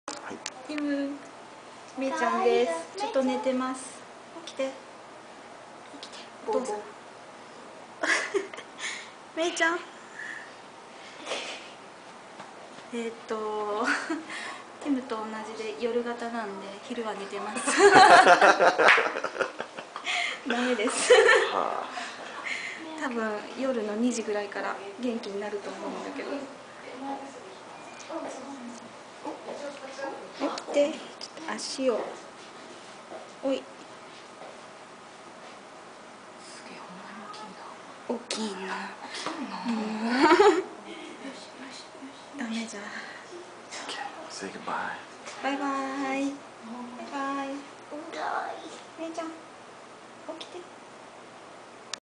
Tim and Ryoko's baby Mei-chan
On Sunday, Ayumi and I stopped by the hospital and visited Ryoko and Mei, she was sleeping the whole time, but very cute.